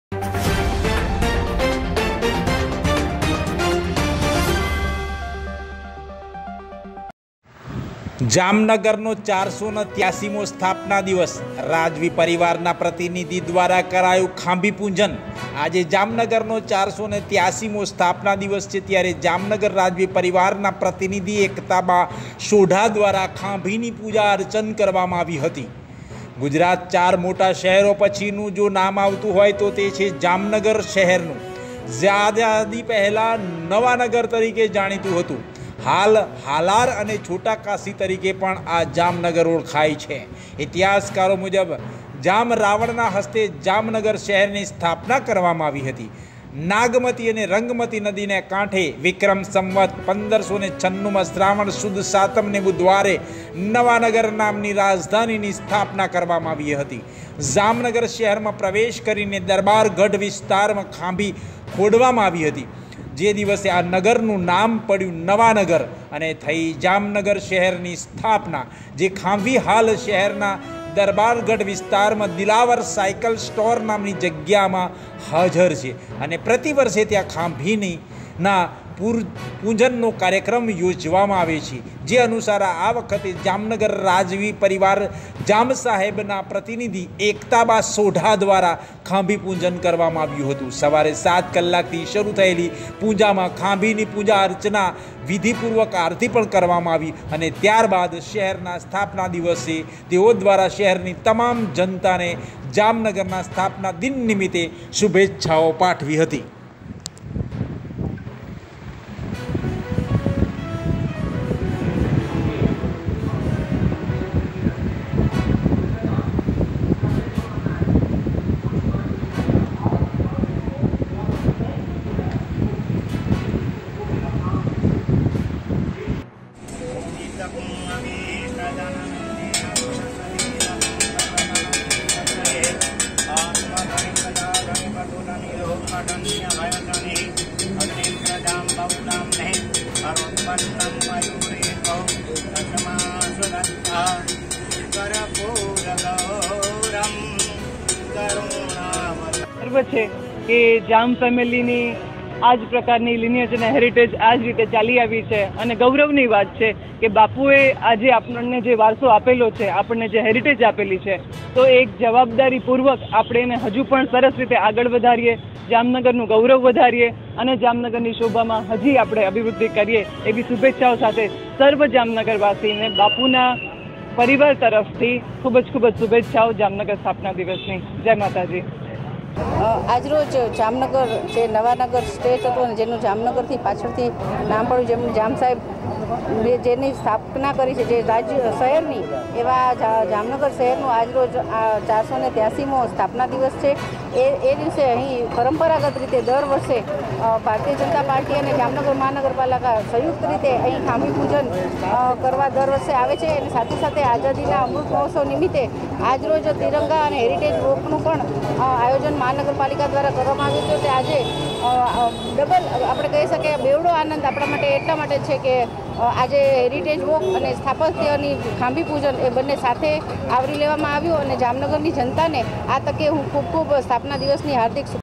જામનગર રાજવી પરિવારના પ્રતિનિધિ એકતાબા સોઢા દ્વારા ખાંભીની પૂજા અર્ચના
𝗟𝗮𝘁𝗲𝘀𝘁 𝗡𝗲𝘄𝘀:જામનગરનો 483મો સ્થાપના દિવસ. રાજવી પરિવારના પ્રતિનિધિ દ્વારા કરાયું ખાંભી પૂજન.